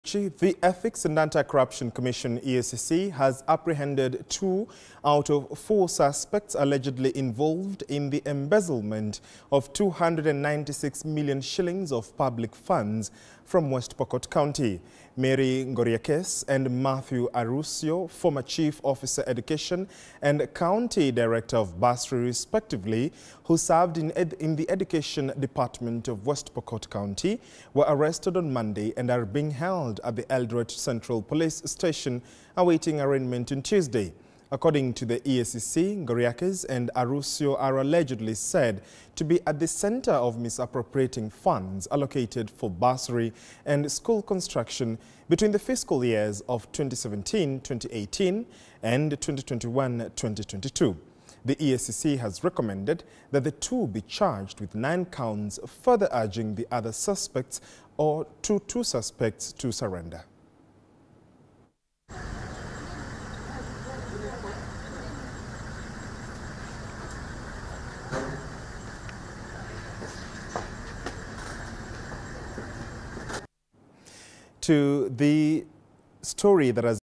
Former West Pokot County officials arrested over graft worth Ksh 296 million
The Ethics and Anti-Corruption Commission (EACC) has apprehended two out of four suspects allegedly involved in the embezzlement of 296 million shillings of public funds from West Pokot County.